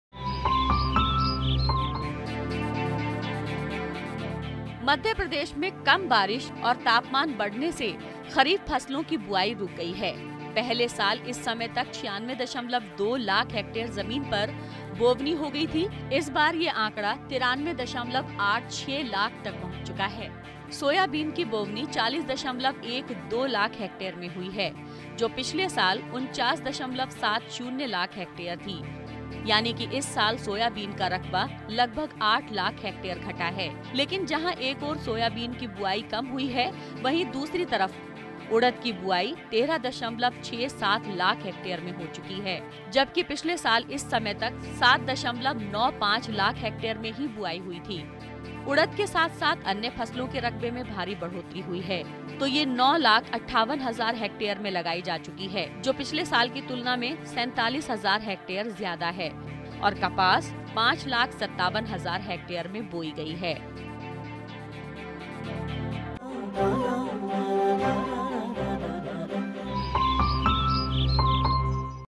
Soyabean के रकबे मे कमी | कुछ फसलों के खरीफ (Kharif Fasalon) की बुवाई मे आई कमी | मध्यप्रदेश
Soyabean के रकबे मे कमी | खरीफ (Kharif Fasalon) की बुवाई मे आई कमी | मध्यप्रदेश